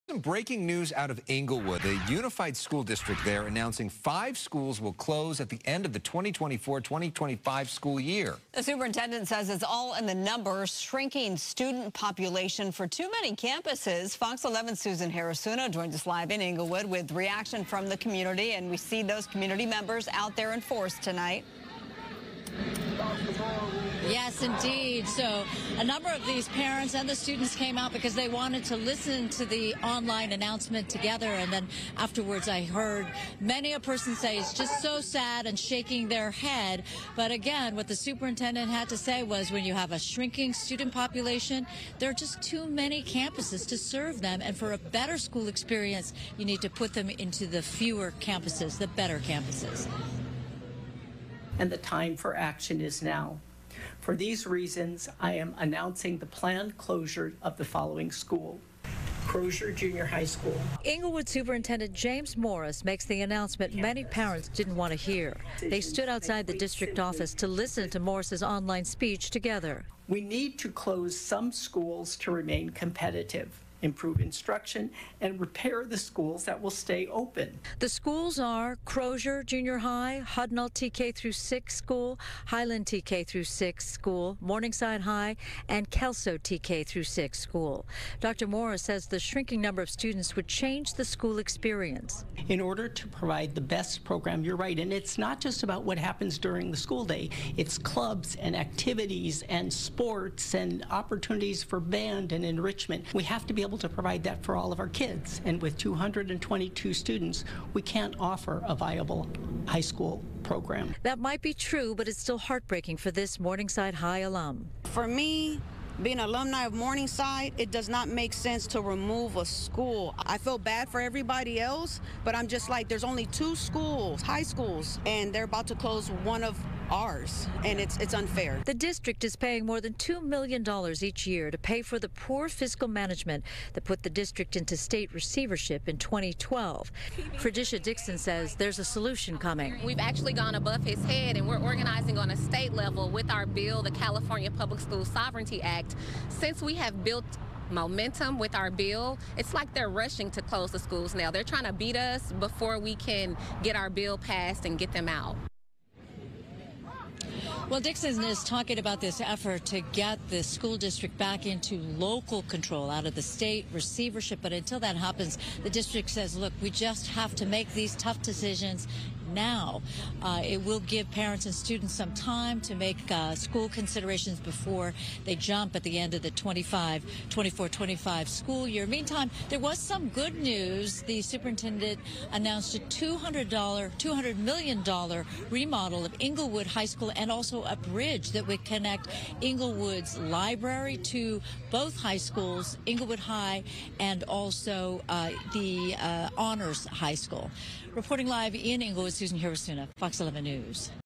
5 Inglewood schools slated to shutdown
Citing a decline in enrollment, Inglewood superintendent said five schools will close at the end of the school year.